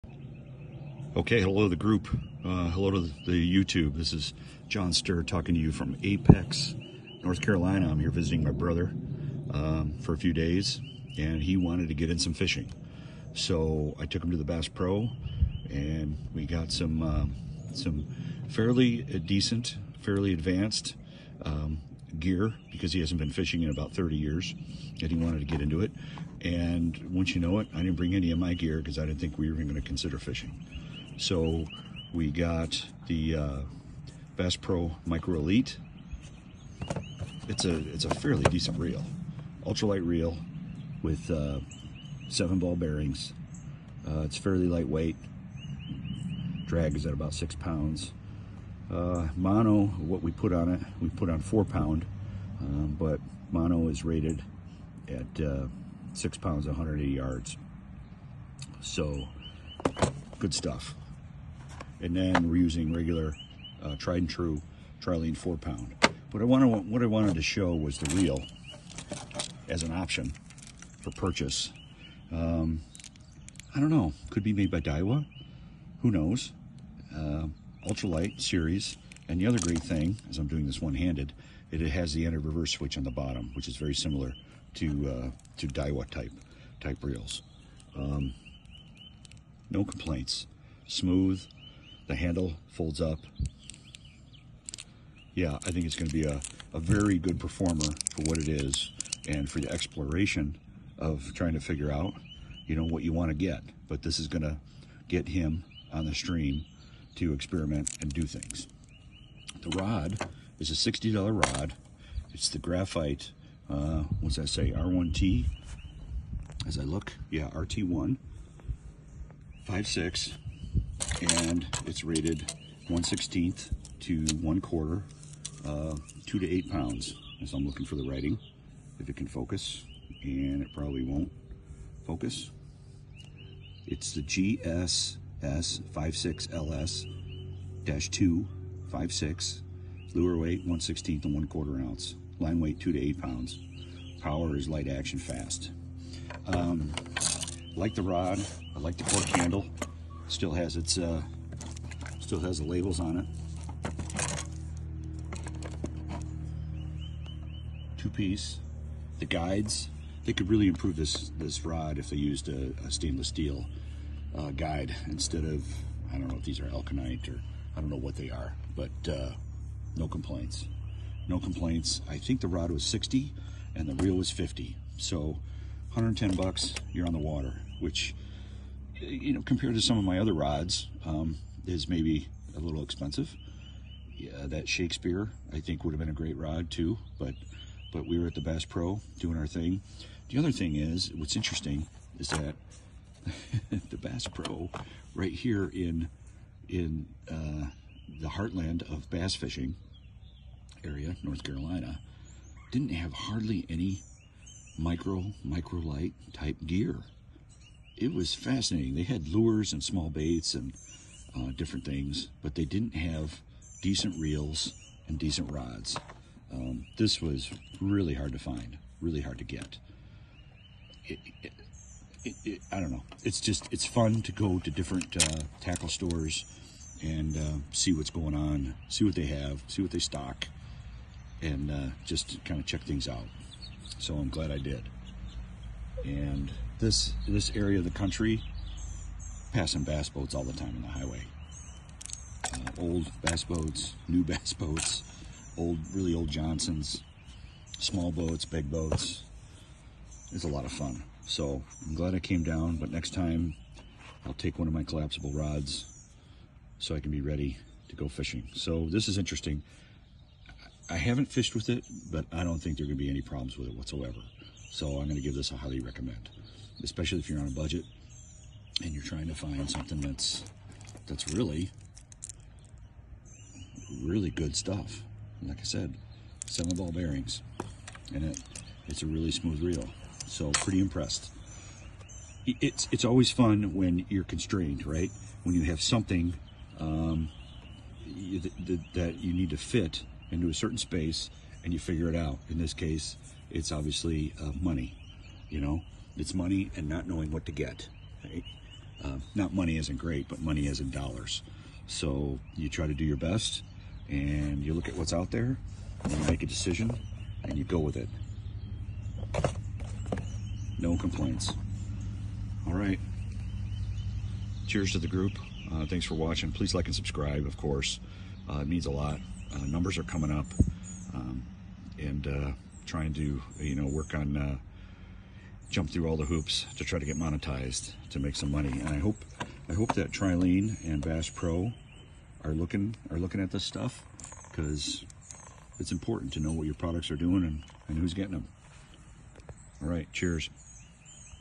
Bass Pro Micro Lite Elite- Spinning Reel
Here’s a Bass Pro Micro Lite Elite Spinning reel and Rod.

Show and Tell